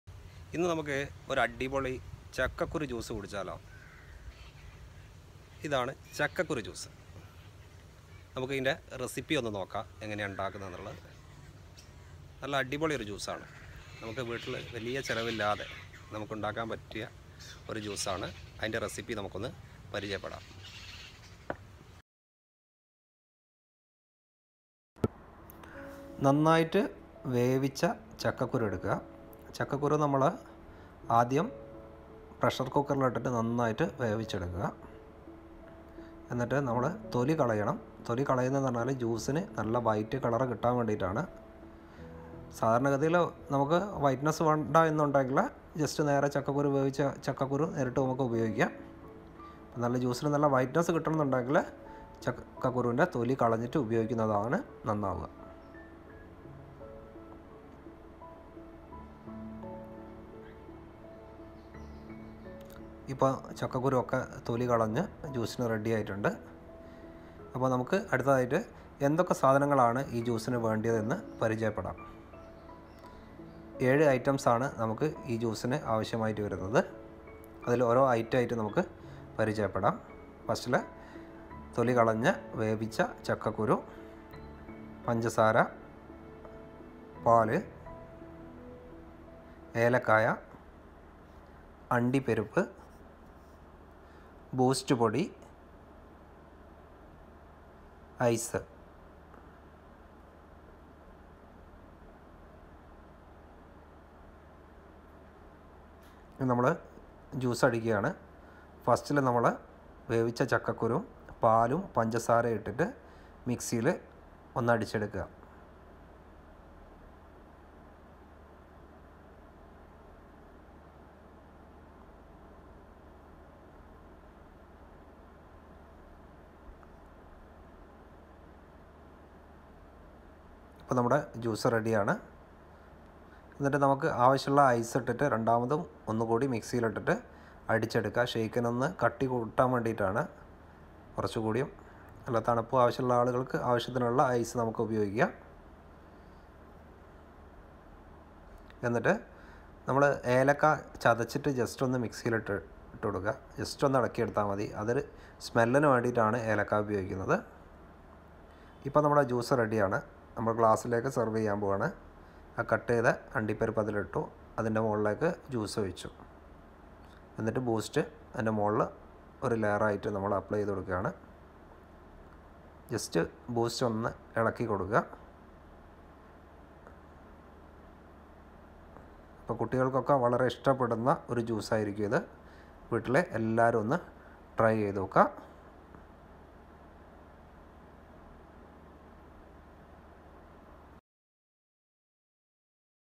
ചക്കക്കുരു ജ്യൂസ് | FAS TALKS
പാലും ചക്കക്കുരുവും ഉപയോഗിച്ചൊരു ജ്യൂസ്